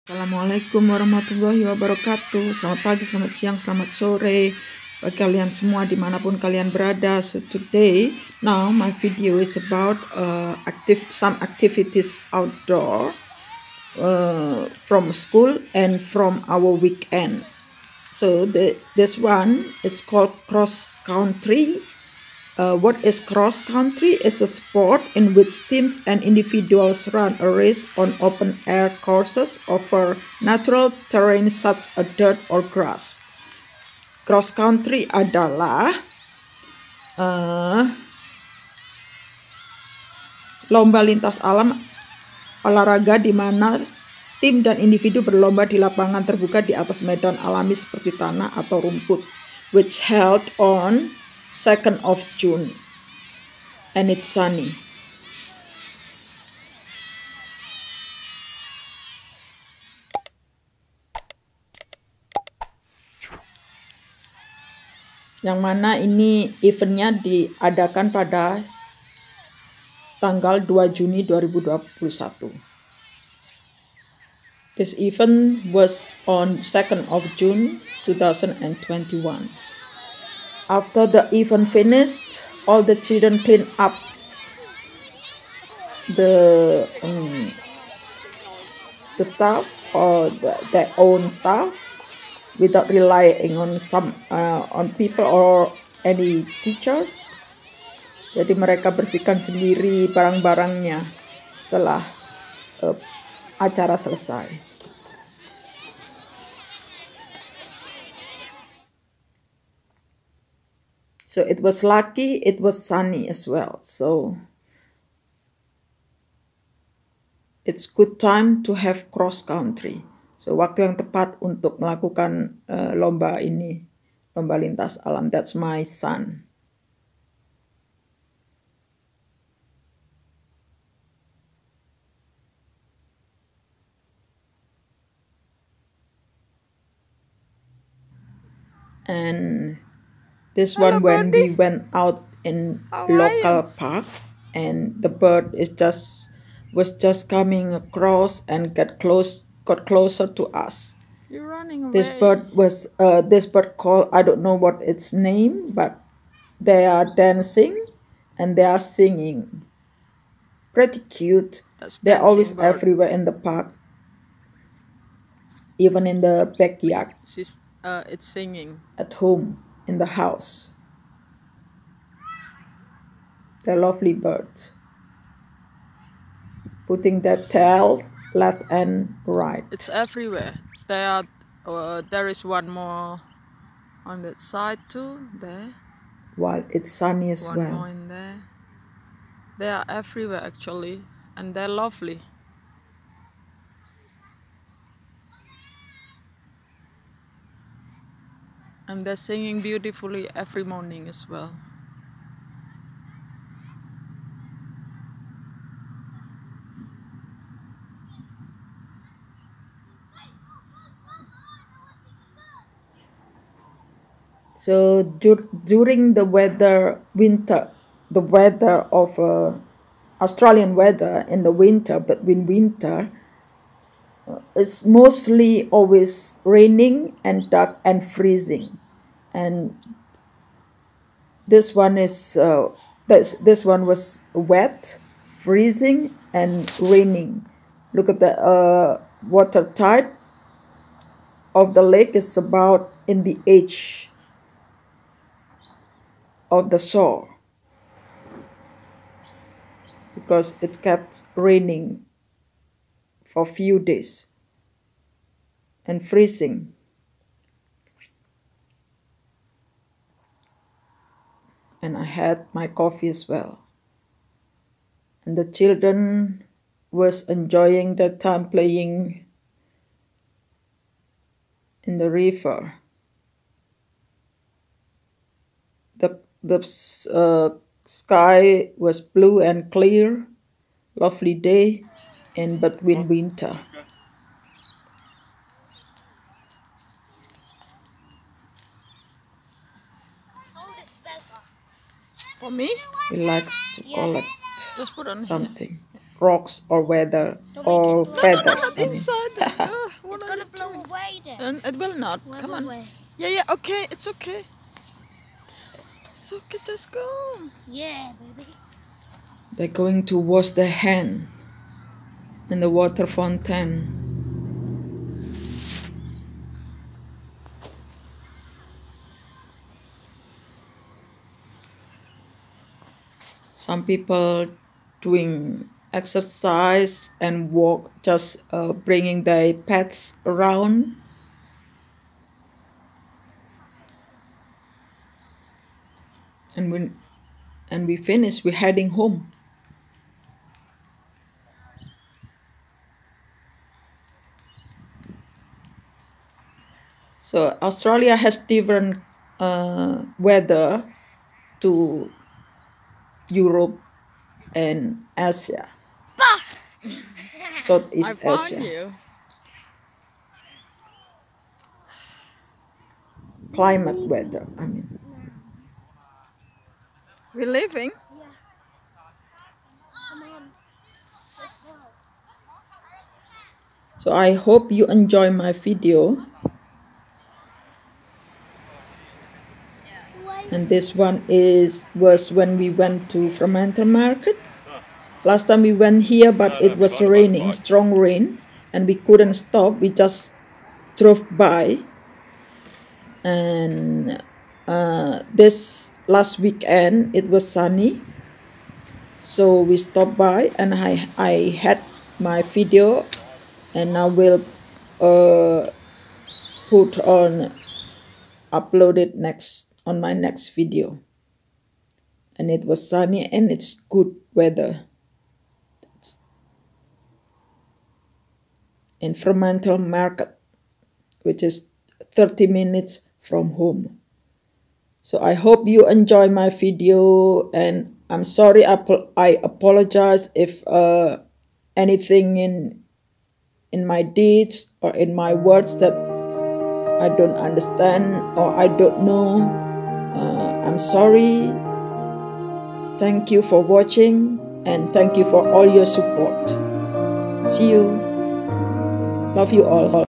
OUTDOOR VLOG: Australia in Between Winter
Beberapa kegiatan di luar rumah disela - sela musim dingin di Australia, salah satunya adalah cross country.

Some outdoor activities in between winter in Australia, one of those is cross country.